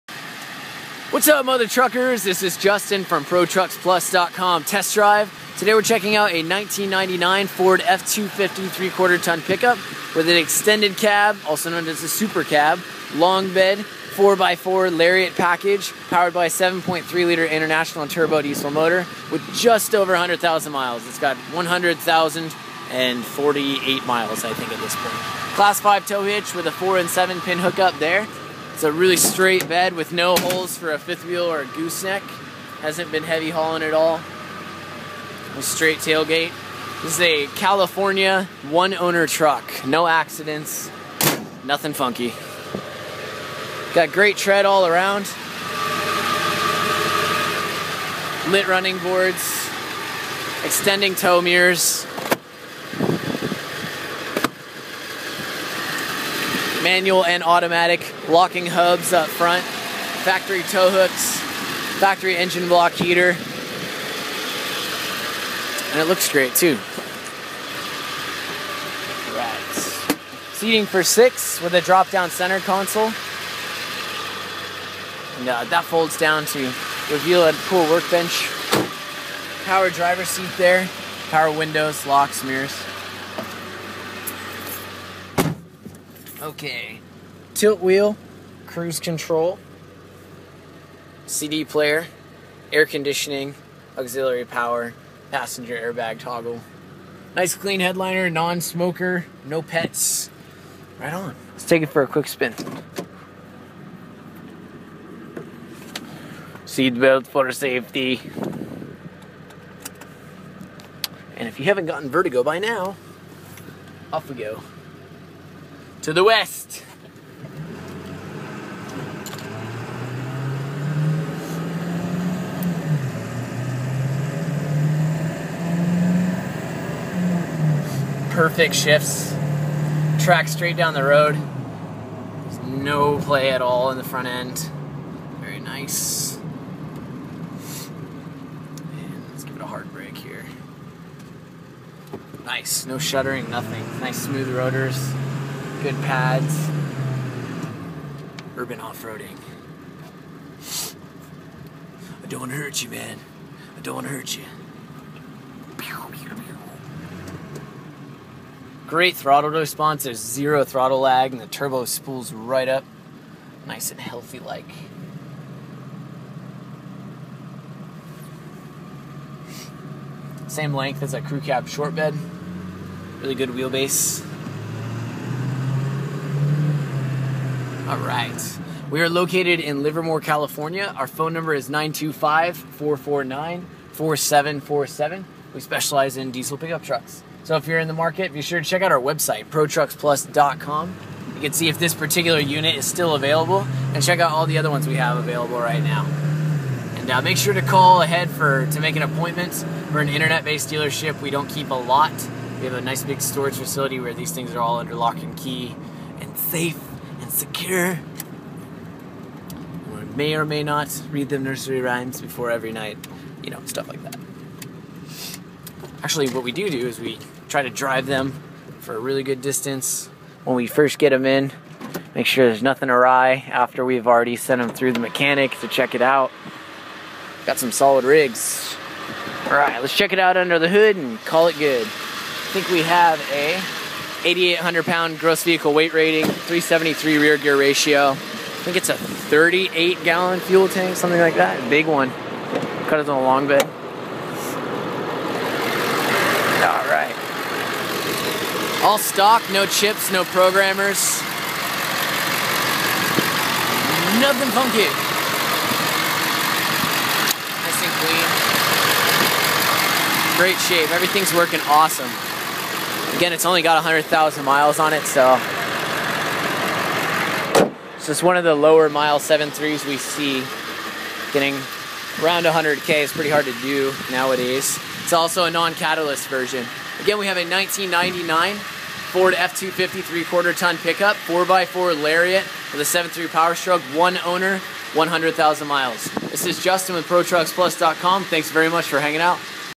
100K MILE 7.3L POWERSTROKE DIESEL 4x4 LARIAT ONE OWNER CALI RIG. No accidents, non smoker
Is your go-to pickup specialty shop! The best part is: NOT A SINGLE AUCTION RIG. Check us out! THANK YOU FOR YOUR TIME 'MURICA!!! Canada too! - Matt